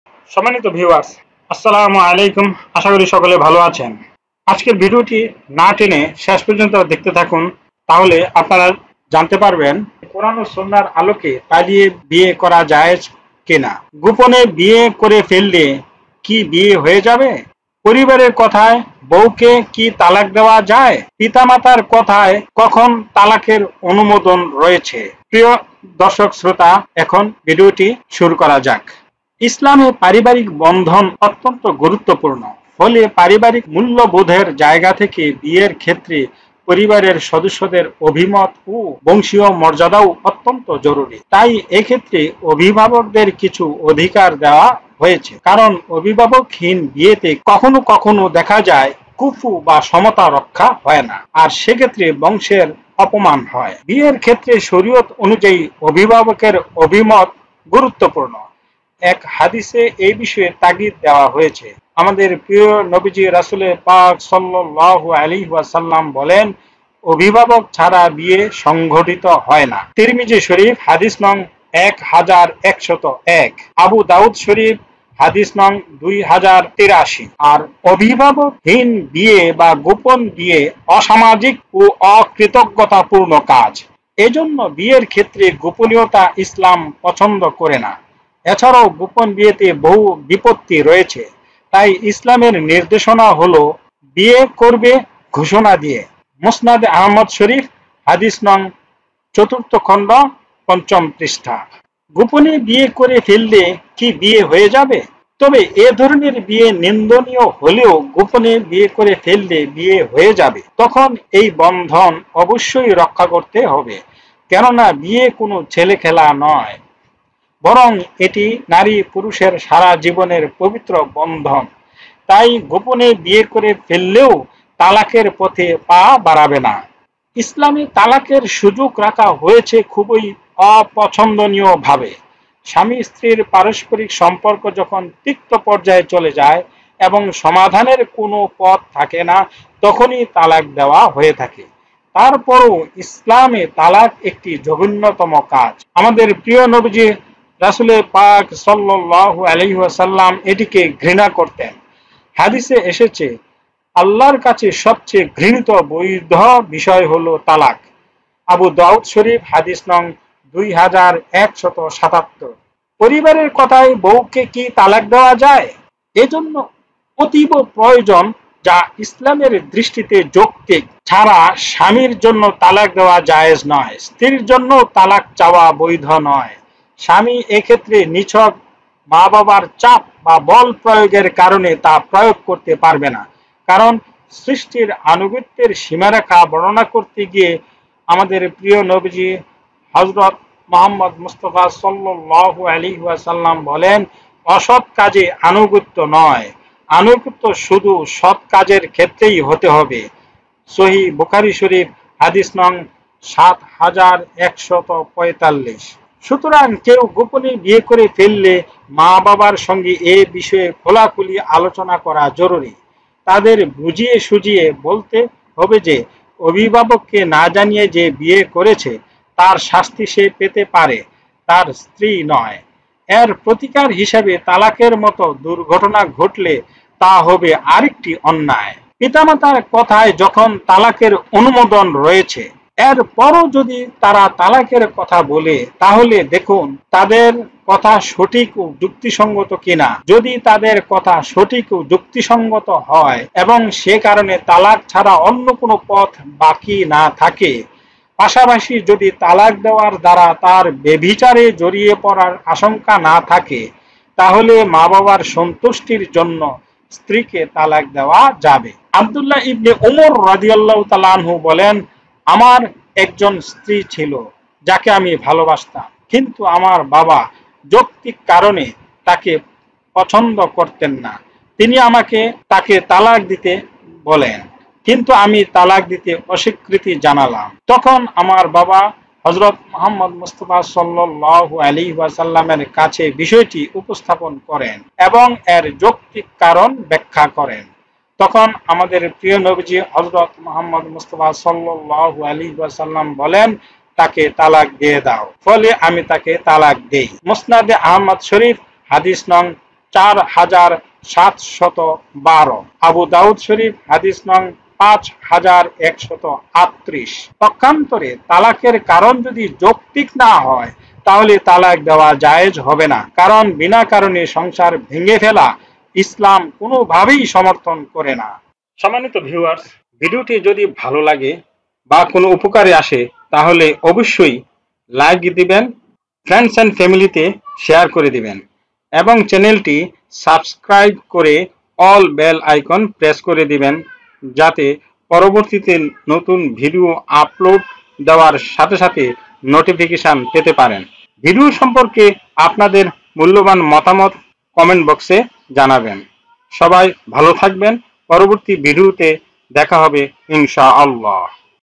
পালিয়ে বিয়ে করা কি জায়েজ | monitor-i
পালিয়ে বিয়ে করা কি জায়েজ | ইসলামে পারিবারিক বন্ধন গুরুত্বপূর্ণ। ফলে পারিবারিক মূল্যবোধের জায়গা থেকে বিয়ের ক্ষেত্রে পরিবারের সদস্যদের অভিমত ও বংশীয় মর্যাদাও অত্যন্ত জরুরি। তাই এ ক্ষেত্রে অভিভাবকদের কিছু অধিকার দেওয়া হয়েছে। কারণ, অভিভাবকহীন বিয়েতে কখনো কখনো দেখা যায়— ‘কুফু’ বা সমতা রক্ষা হয় না। আর সে ক্ষেত্রে বংশের অপমান হয়।
বিয়ের ক্ষেত্রে শরিয়ত অনুযায়ী অভিভাবকের অভিমত গুরুত্বপূর্ণ। এক হাদিসে এ বিষয়ে তাগিদ দেওয়া হয়েছে। আল্লাহর রাসুল (সা.) বলেন, ‘অভিভাবক ছাড়া বিয়ে সংঘটিত হয় না।’ (তিরমিজি, হাদিস : ১১০১; আবু দাউদ, হাদিস : ২০৮৩)

আর অভিভাবকহীন বিয়ে বা গোপন বিয়ে অসামাজিক ও অকৃতজ্ঞতাপূর্ণ কাজ। এ জন্য বিয়ের ক্ষেত্রে গোপনীয়তা ইসলাম পছন্দ করে না। এছাড়াও গোপন বিয়েতে বহু বিপত্তি রয়েছে। তাই ইসলামের নির্দেশনা হলো, ‘বিয়ে করবে- ঘোষণা দিয়ে।’ (মুসনাদে আহমাদ, হাদিস : ৪/৫)